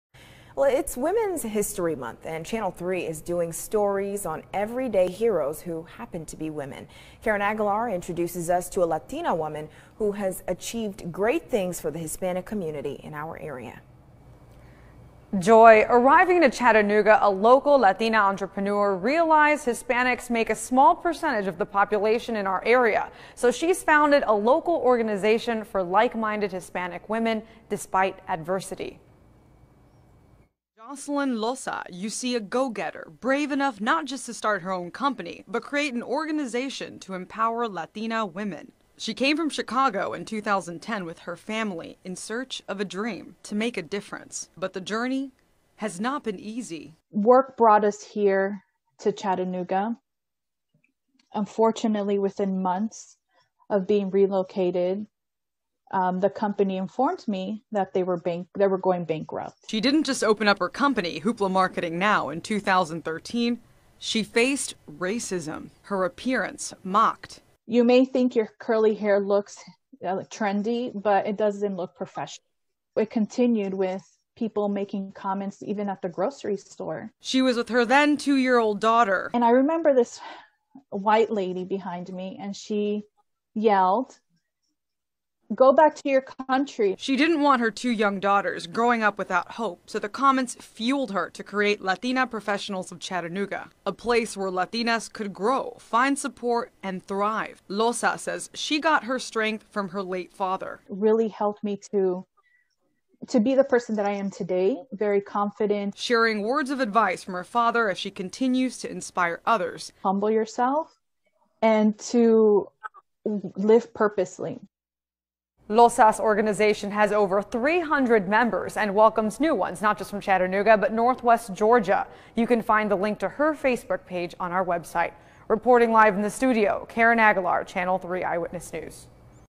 Entrepreneur Latina creates empowerment organization for Hispanic women
Looking at Jocelyn Loza you see a go-getter brave enough not just to start her own company but create an organization to empower Latina women.